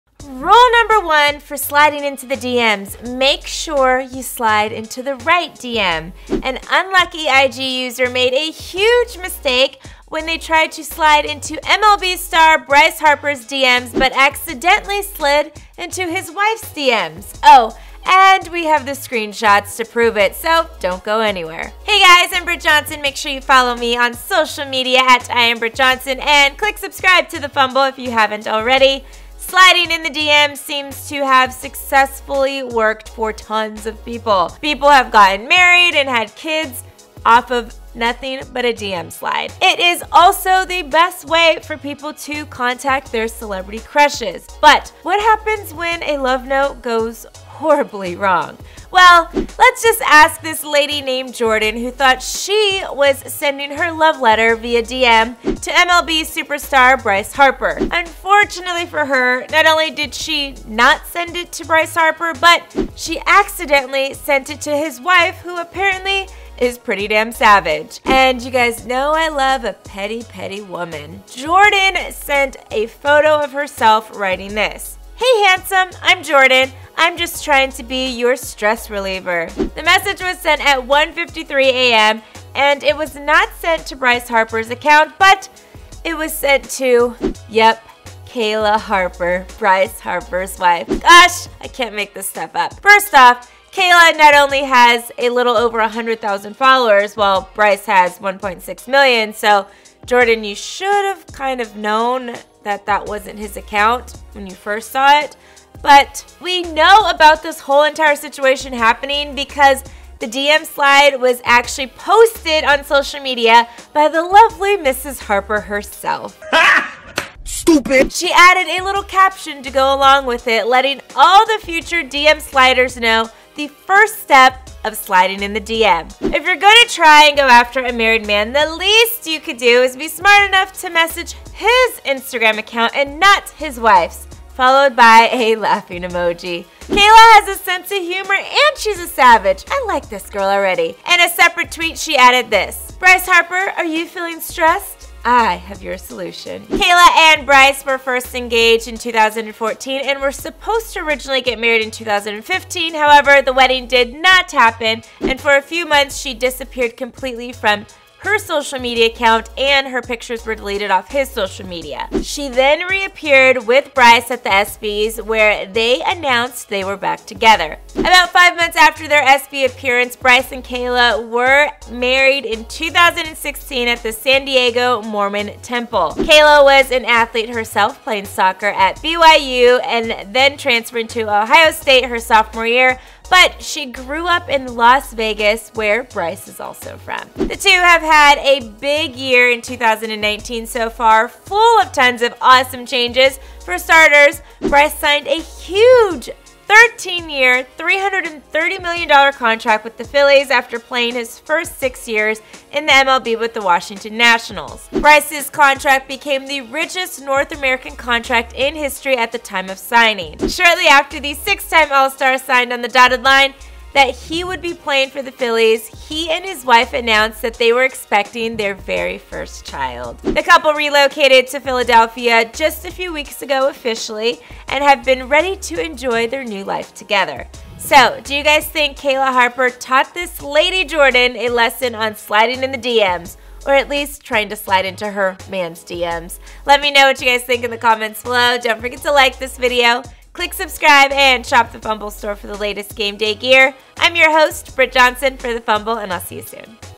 Insta Model Gets CALLED Out For ACCIDENTALLY Sending Thirsty DM To Bryce Harper's Wife!
Rule number one of sliding into the dms make sure you slide into the right dm. An unlucky IG user made a huge mistake when they tried to slide into MLB star Bryce Harpers dms but accidentally slide into his wifes dms.